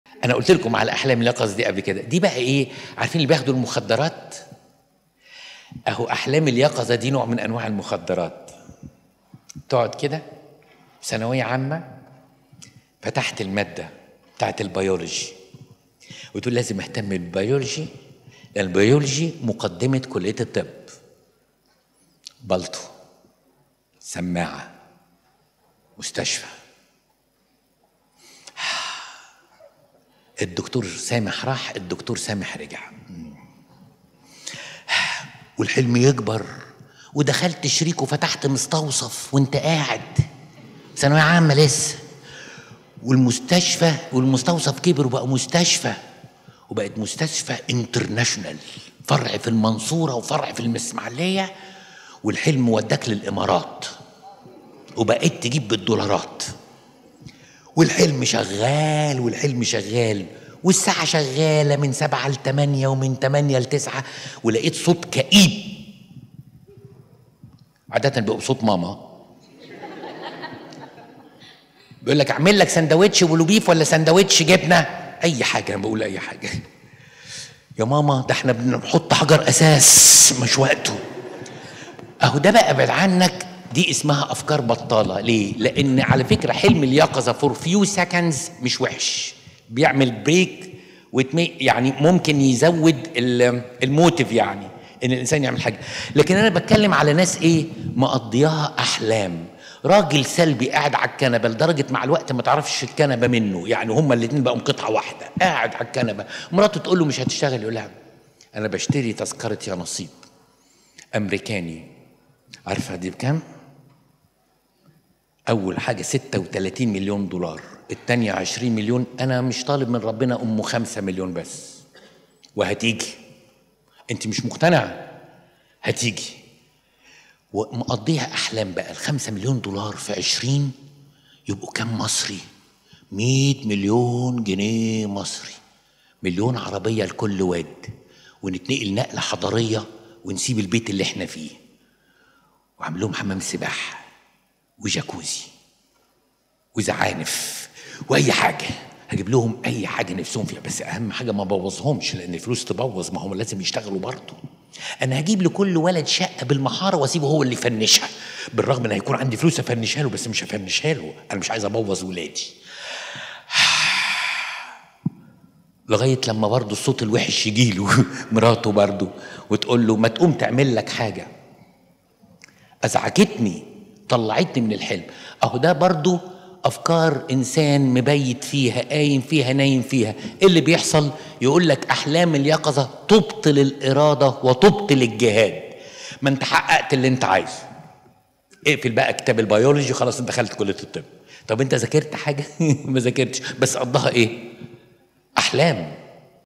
أحلام القظة نوع من الأفكار البطالة!
👈برنامج "الراعي الصالح" مع القمص بولس چورچ
يمكنكم الآن مشاهدة قناة Mesat
على التردد
11603 | ١١٦٠٣
Horizontal | أفقي
27500 | ٢٧٥٠٠
3/4 | ٣/٤